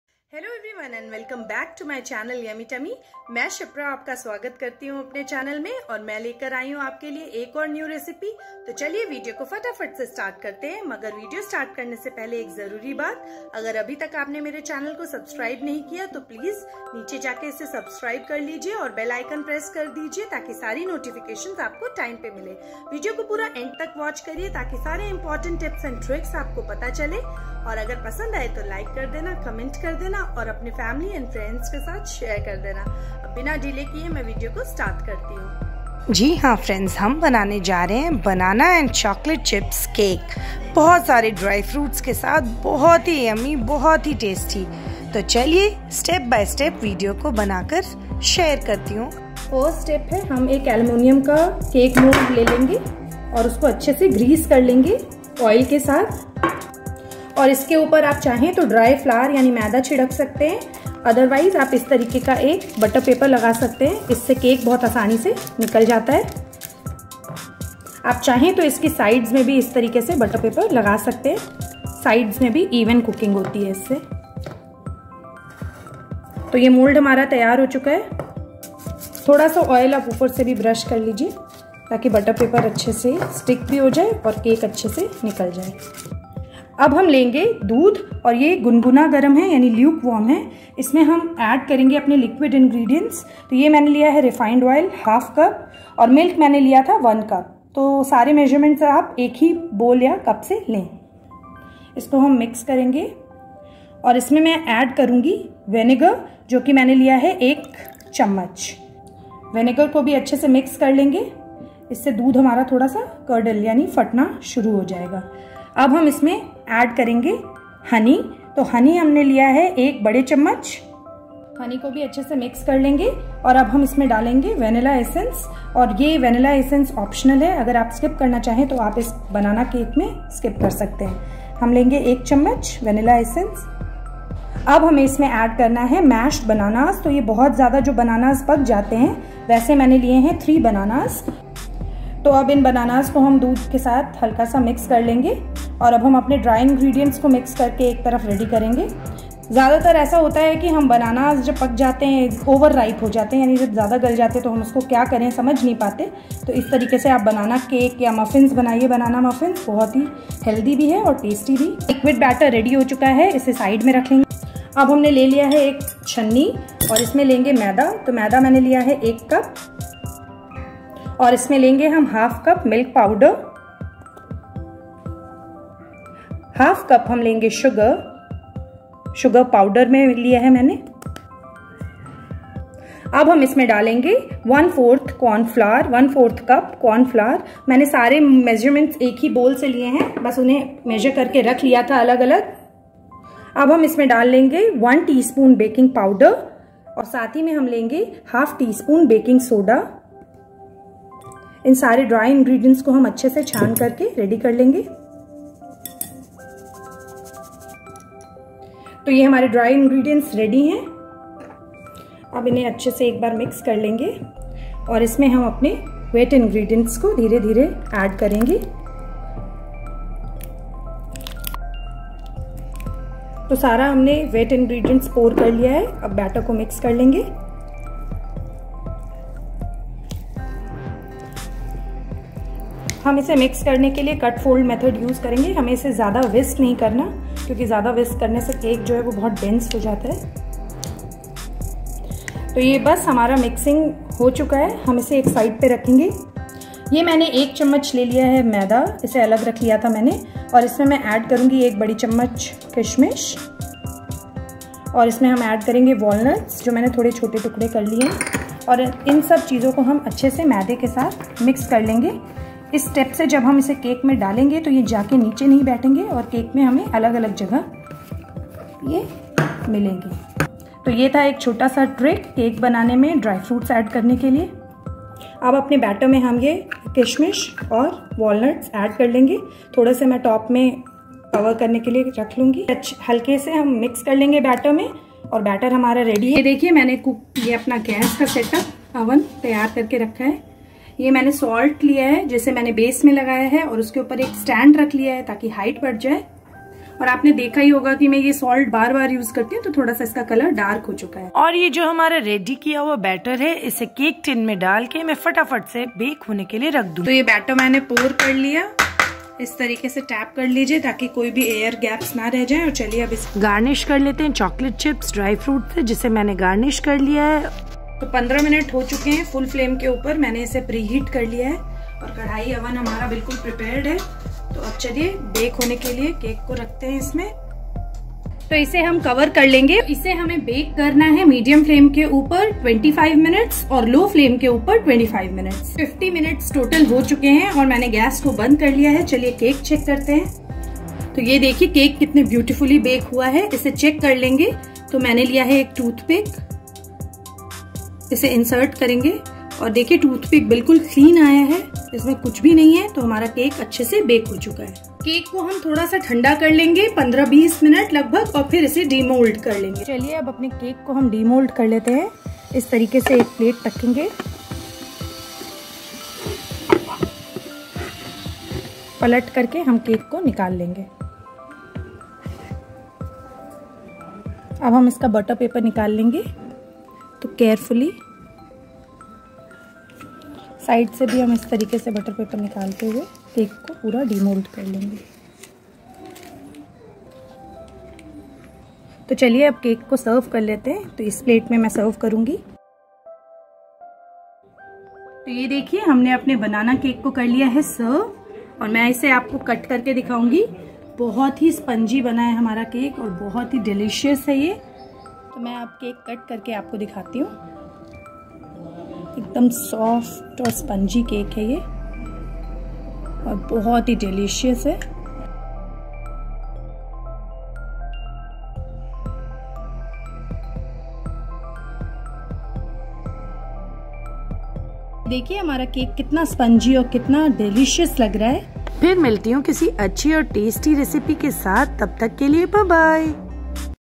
Banana Cake | Banana & Chocolate Chips Cake Recipe | Healthy Cake
Yummiest Banana Cake loaded with lots of chocolate chips and dry nuts ! Yes friends,  aaj mei aapke liy le k aai hu ek easy and tasty Banana cake ki recipe, Is cake ko banana vahut aasan hai aur sagh hi ye ek healthy cake hai.

Links of some products I use :

Rectangular Glass Dish

Barfi Aluminium tray

Green Food Colour

Almonds

Milk Powder

Desi Ghee ( 1/2 kg )

Desi Ghee (1 kg )